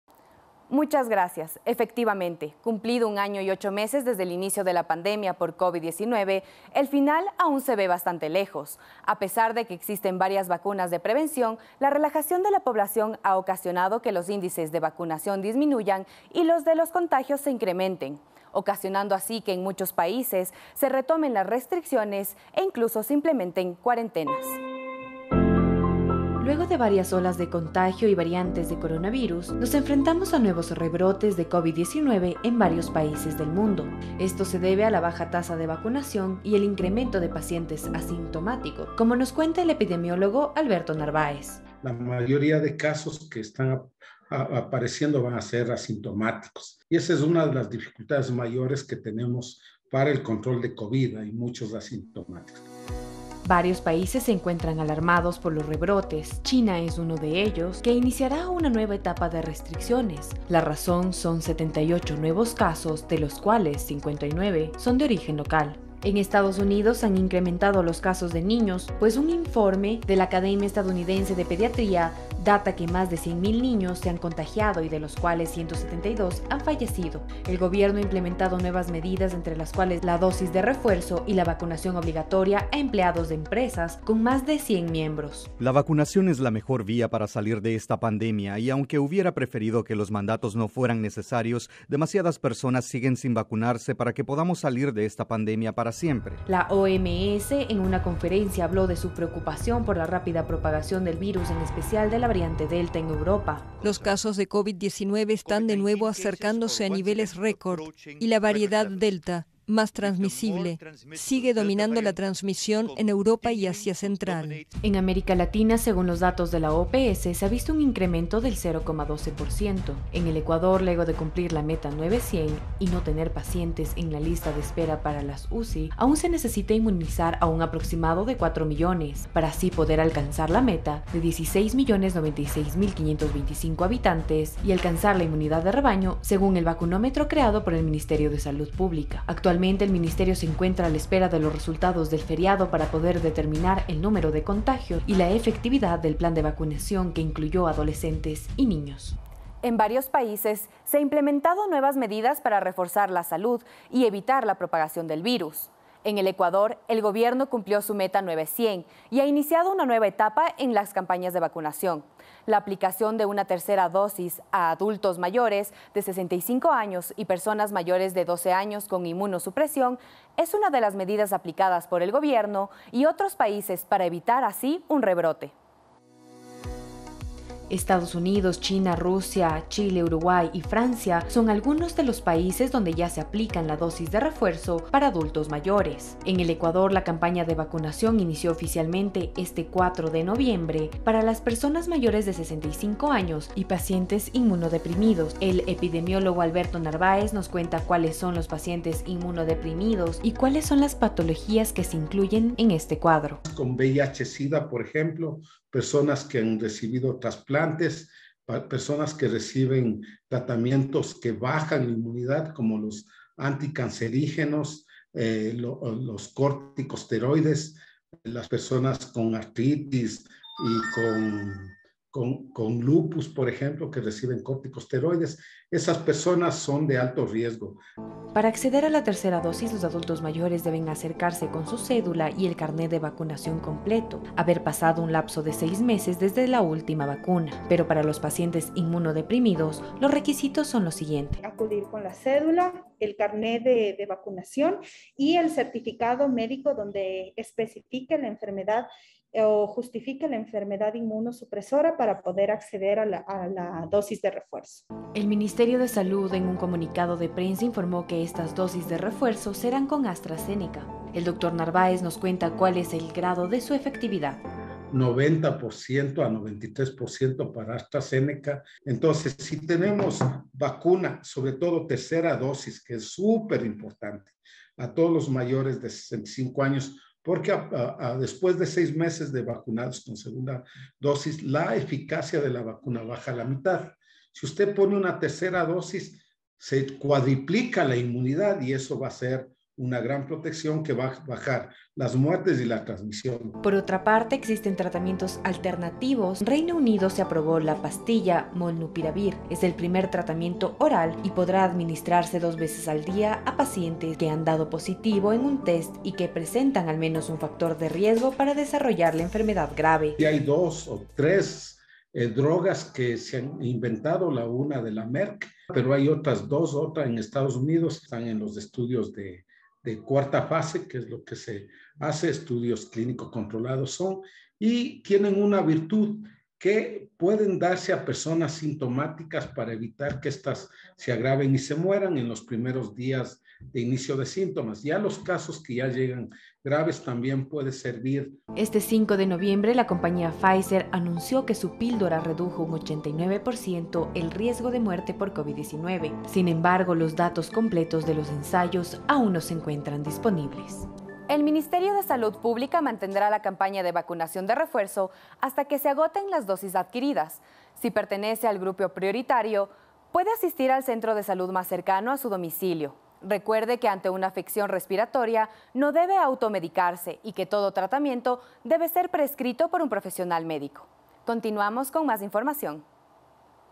Tercera dosis covid-19
Tercera dosis y nuevos tratamientos para el tratamiento del covid-19